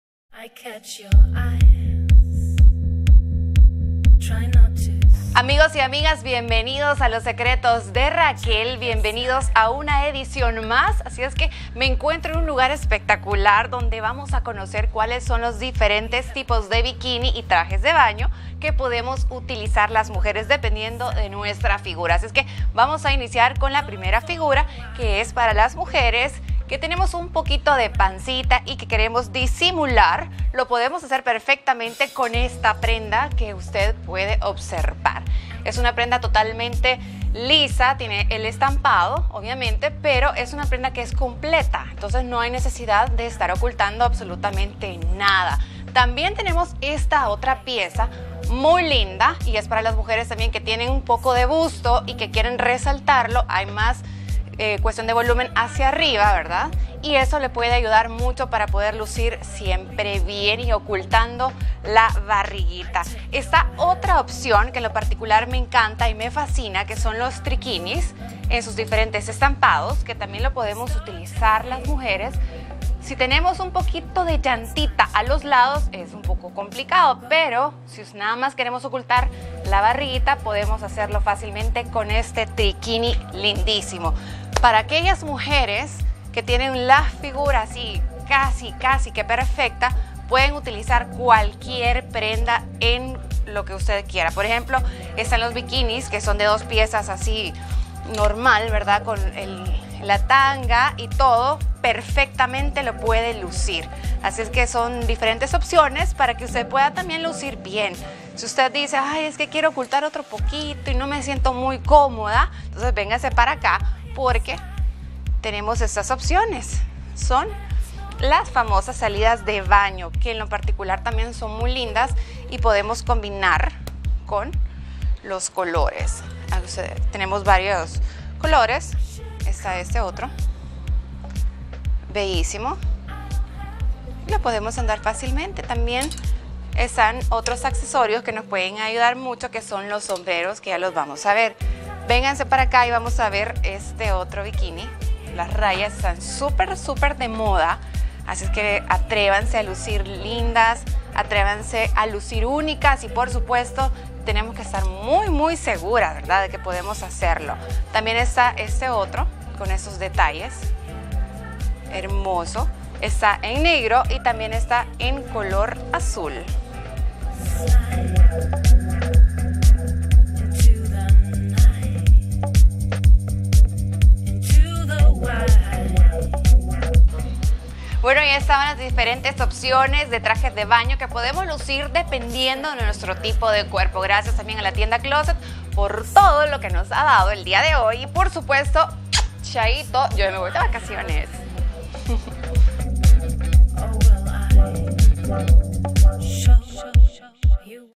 Secretos de Raquel
Tipo de bikini ideal para nuestro cuerpo.